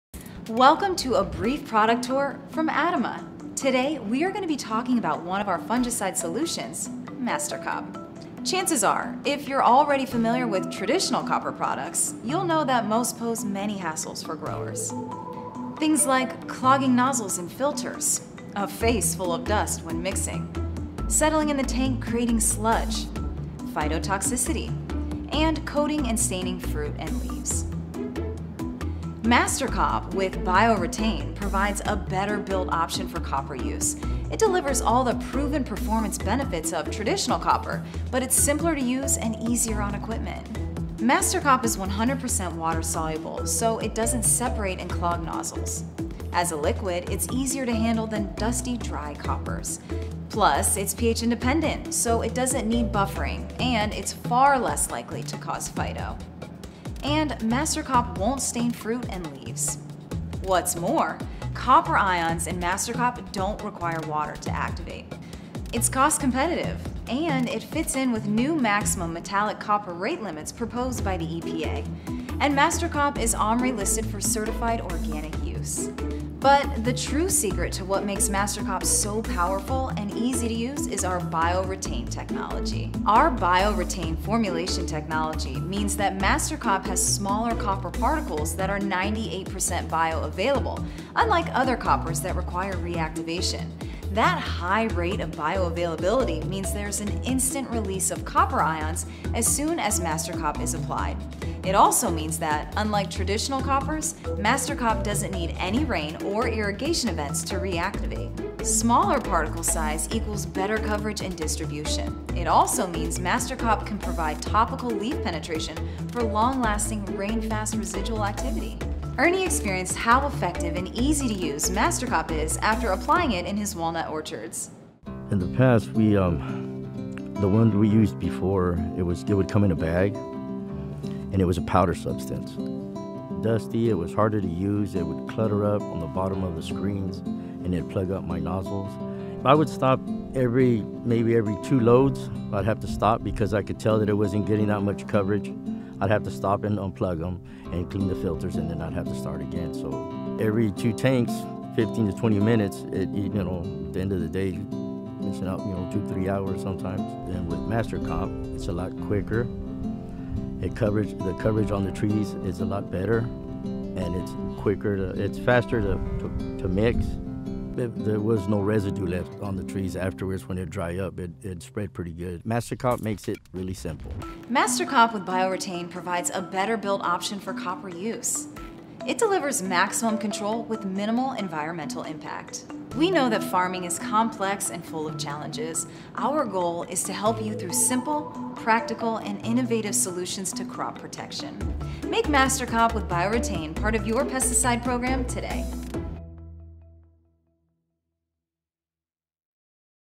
Mastercop, el maestro del control
Nuevo fungicida y bactericida de la línea Advanced, de acción preventiva, amplio espectro y buena persistencia.
Excelente respuesta en frutales que lo utilizan como nutriente. Su tecnología Bio Retain le permite una mayor biodisponibilidad del cobre en el cultivo, lo que asegura un control eficiente de hongos y bacterias.